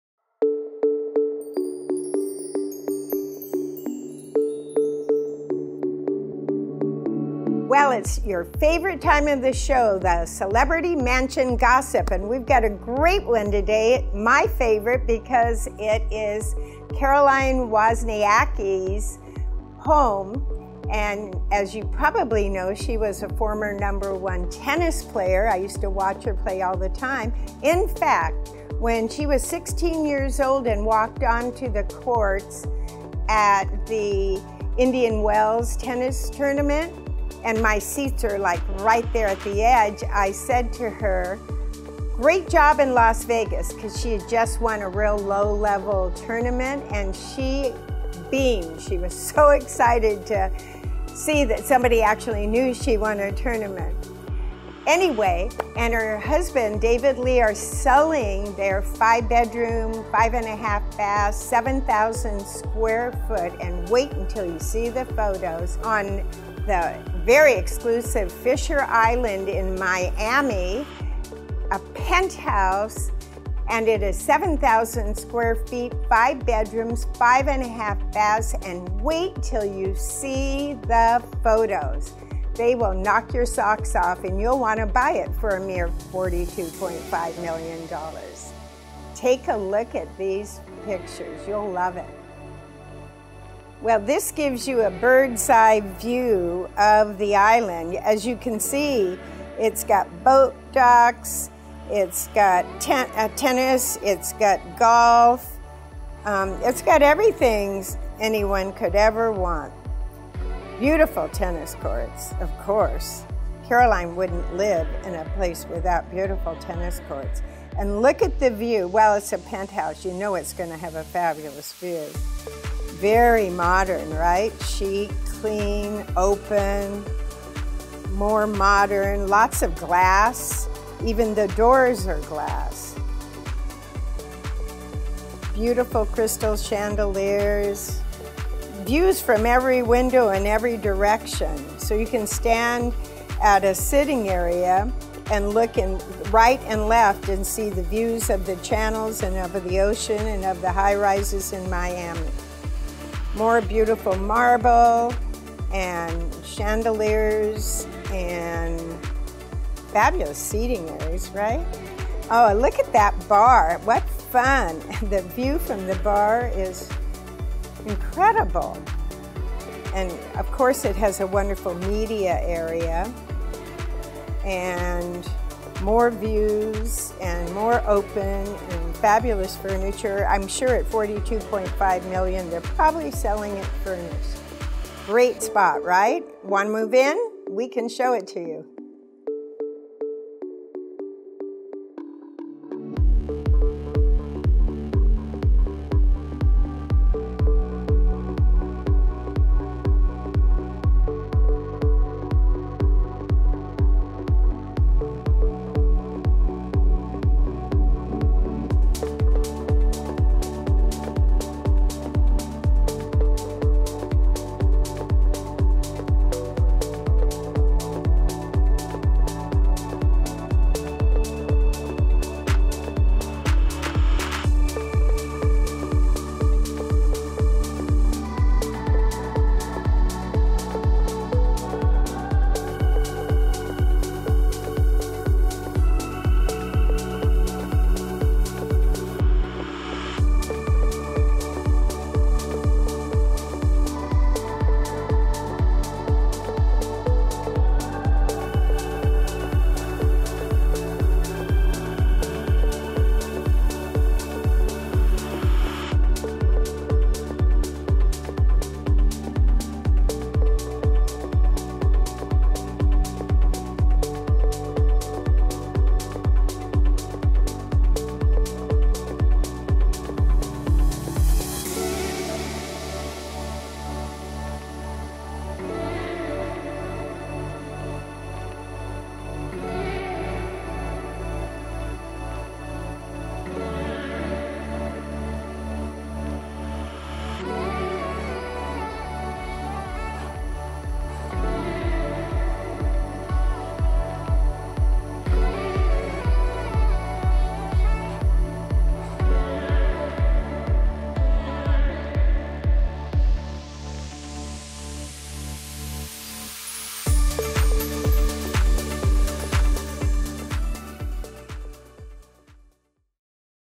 Lee Ann's Celebrity Mansion Gossip - Caroline Wozniacki & David Lee 🎾🏀
Lee Ann Canaday shares the latest and hottest Celebrity Real Estate news.